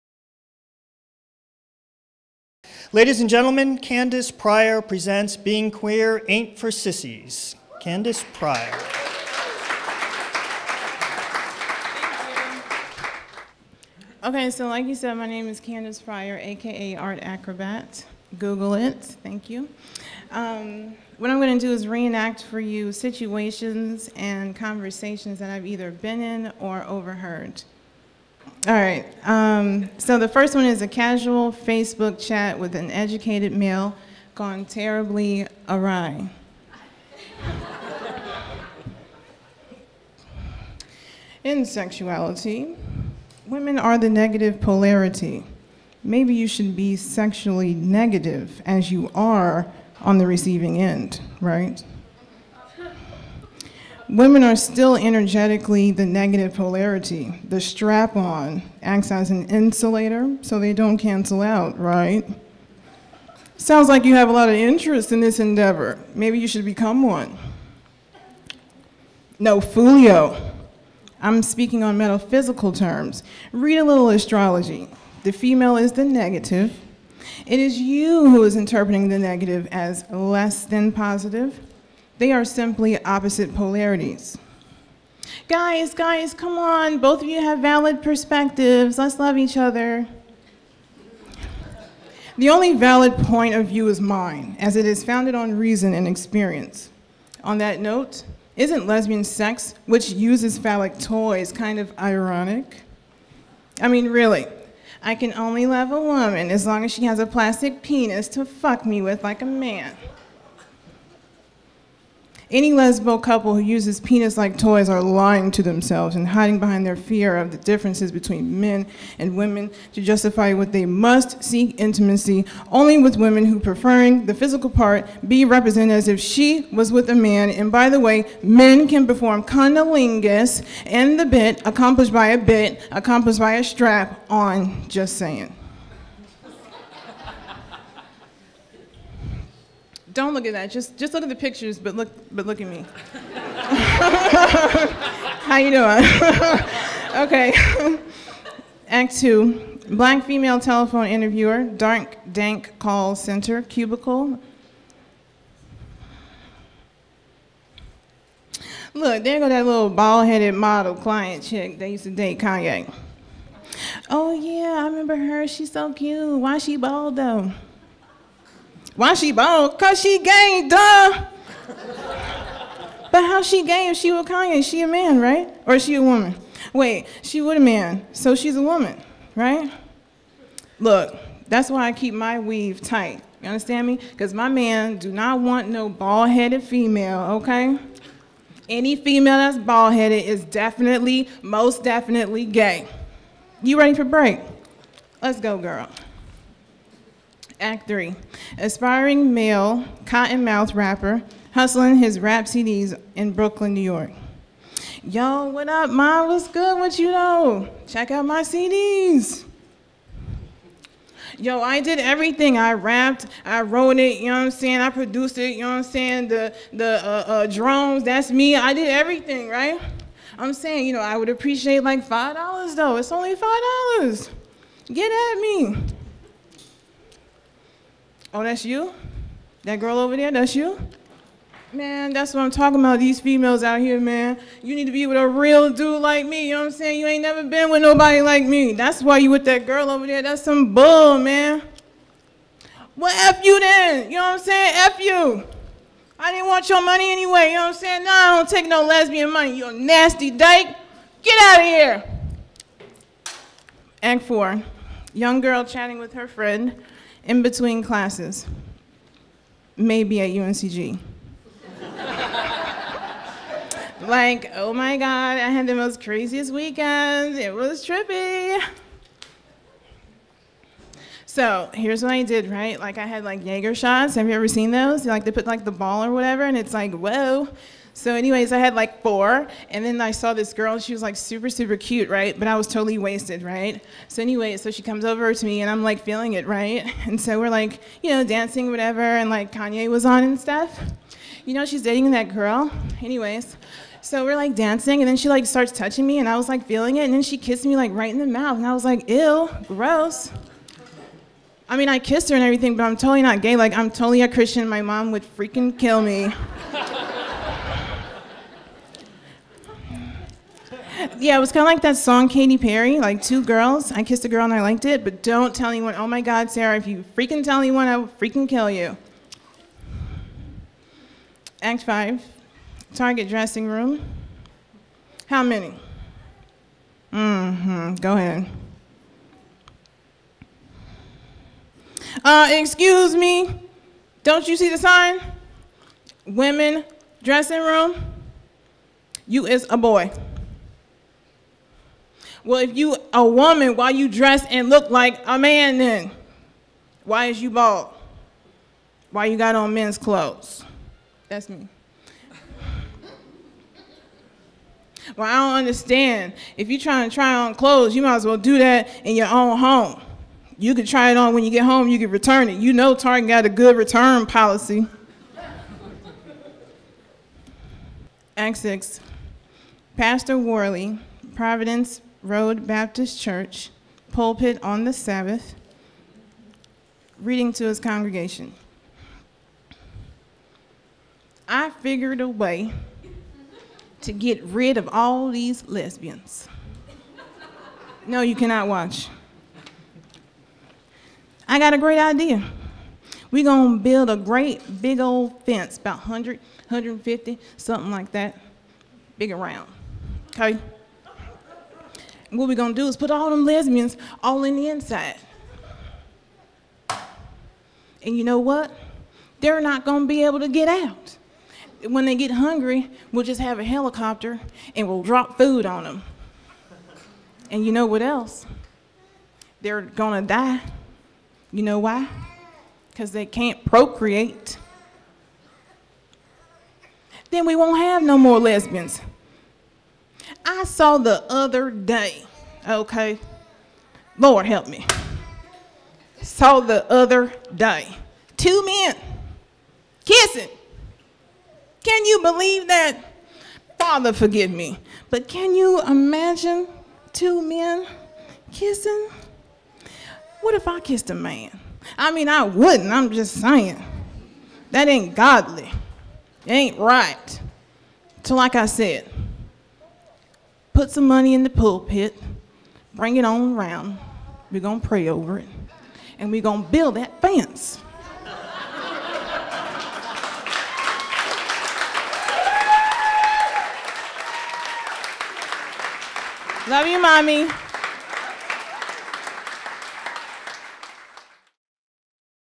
Candace Pryor- Being Queer Ain't for Sissies!
PechaKucha Raleigh Vol. 11

Visual Art Exchange, Raleigh, NC